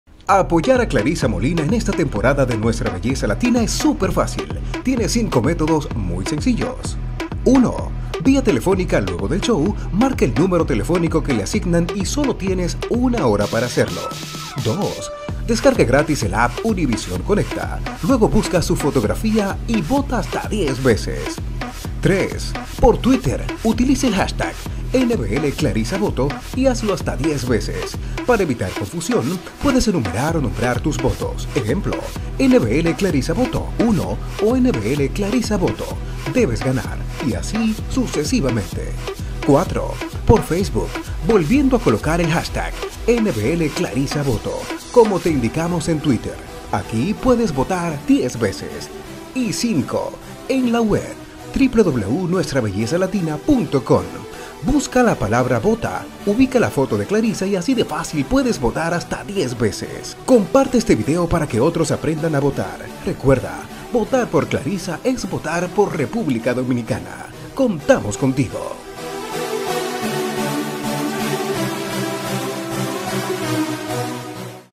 COMO VOTAR POR CLARISSA MOLINA NBL 2016
Clarissa Molina es una modelo profesional, actriz, presentadora de televisión y reina de belleza dominicana. Participó en el reality de Univision Nuestra Belleza Latina 2015, ​ donde quedó como tercera finalista. En el mismo año fue coronada Miss República Dominicana 2015, ​ donde representó la provincia de Espaillat.